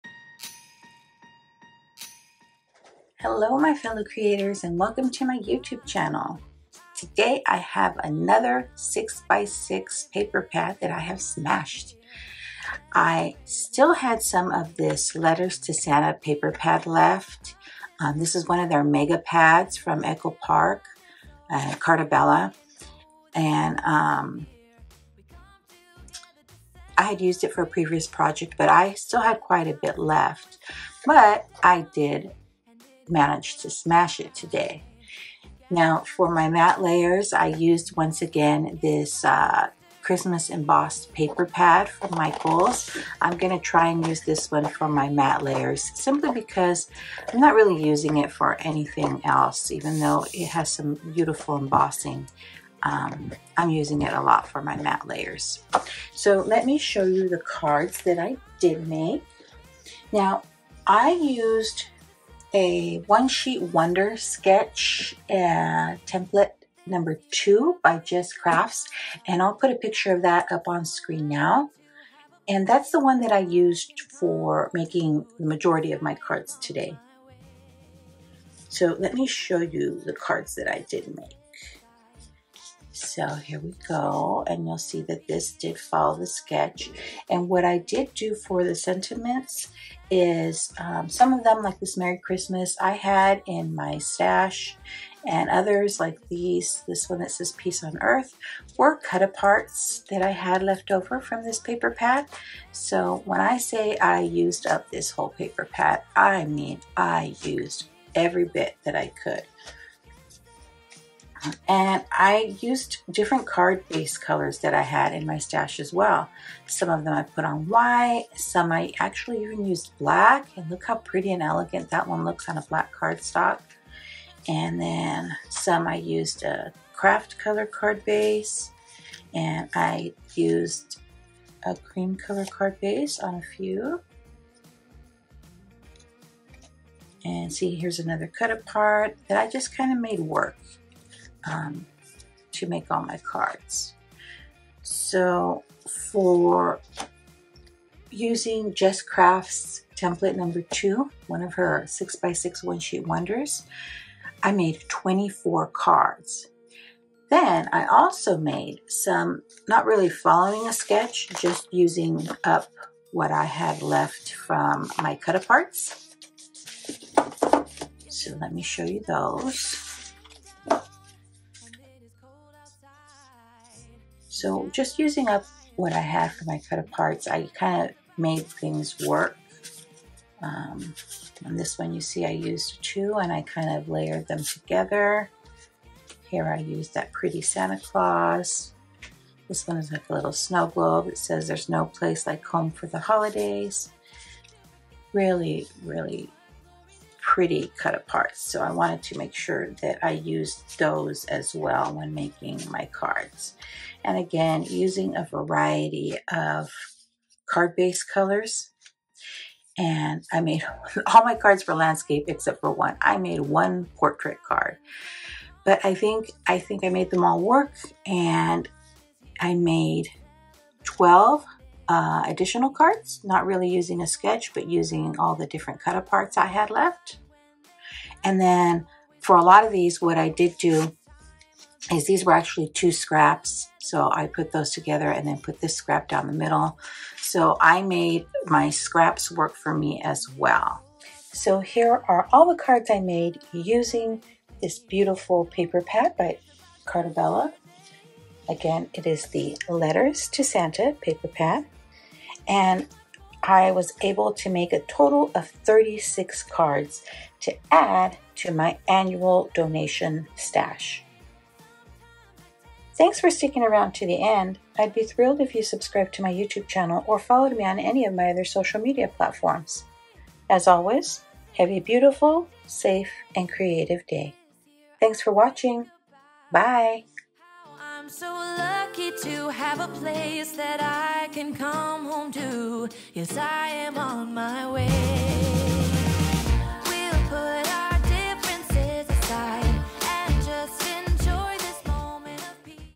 I did it! I smashed another 6x6 paper pad!
I smashed another 6x6 paper pad today. I am well on my way to building up my holiday card stash for my annual donation!